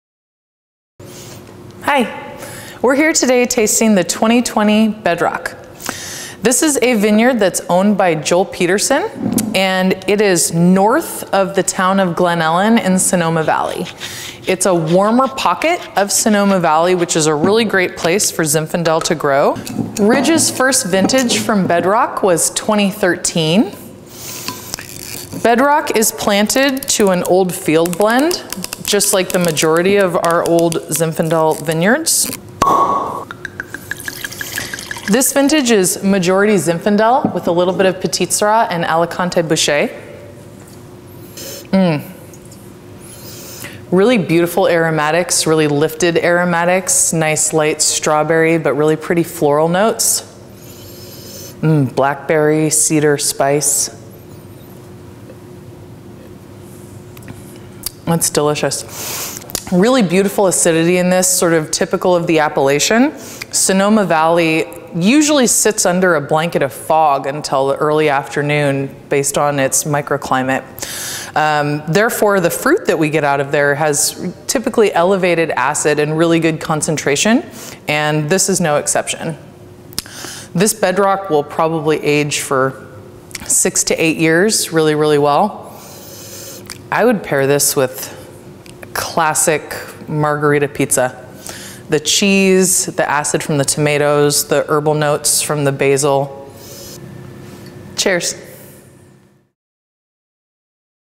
2020 BEDROCK ZINFANDEL
Watch Lytton Springs Winemaker Shauna Rosenblum taste and talk about the 2020 Bedrock Zinfandel.